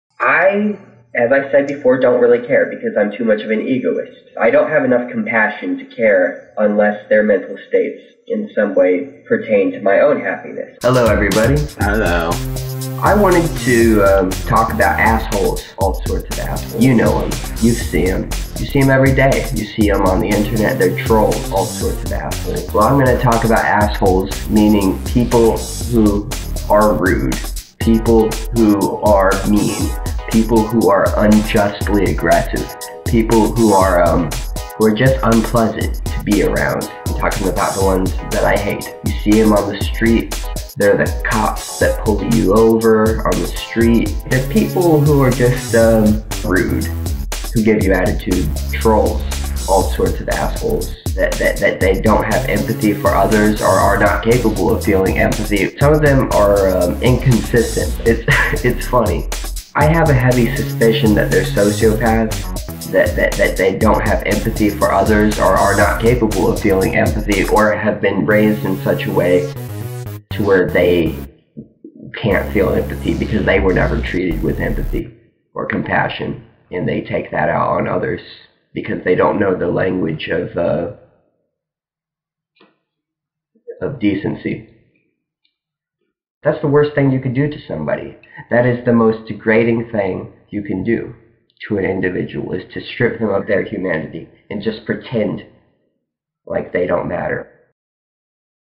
Talk About Assholes...
This isn't intended to be an attack on the individual featured, this video speaks to a larger issue of how people who clearly have, and value, empathy... tend to [knee-jerkily] throw empathy and ethics out of the window upon being presented with antinatalism.